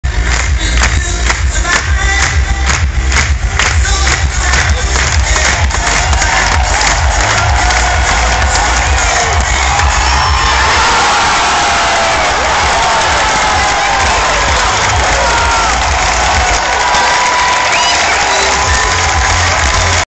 Stabhochsprung Deutscher Rekord 6.01m Björn Otto
Björn Otto Deutscher Rekord 6.01m Aachen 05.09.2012
Stabhochsprung Pole vault